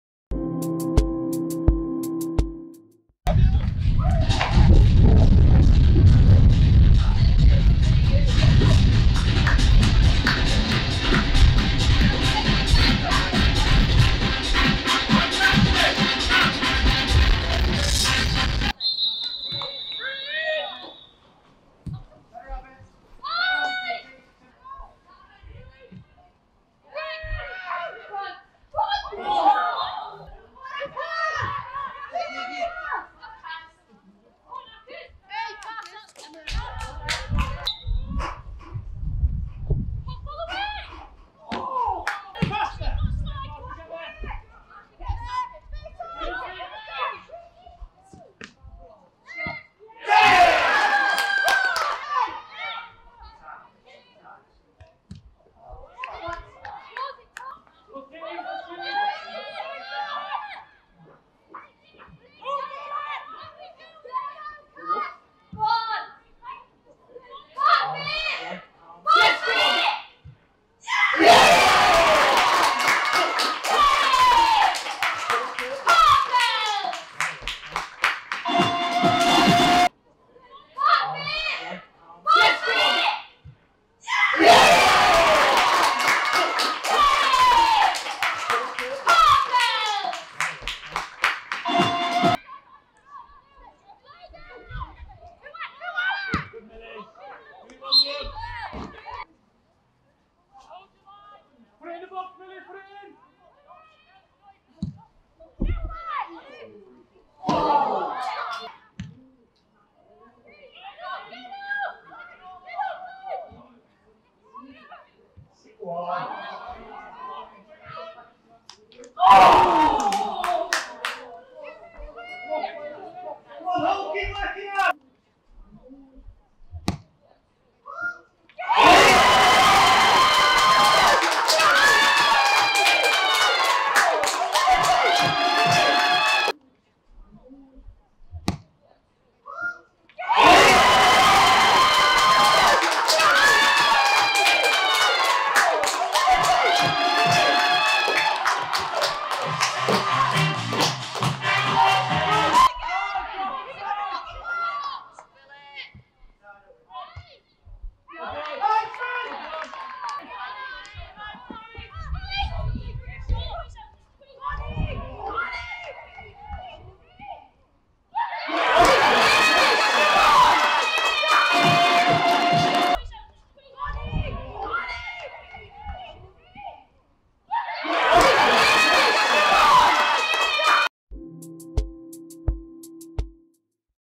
Highlights: Middlesbrough Women 3-1 Hull City Ladies - Vitality Women's FA Cup
Catch all the best bits from today's 3-1 win over Hull City Ladies in today's Vitality Women's FA Cup second round tie.

Goals from Faye Dale, Millie Bell and Eve Marshall cancelled out Katie Thompson's opener to send Boro into round three.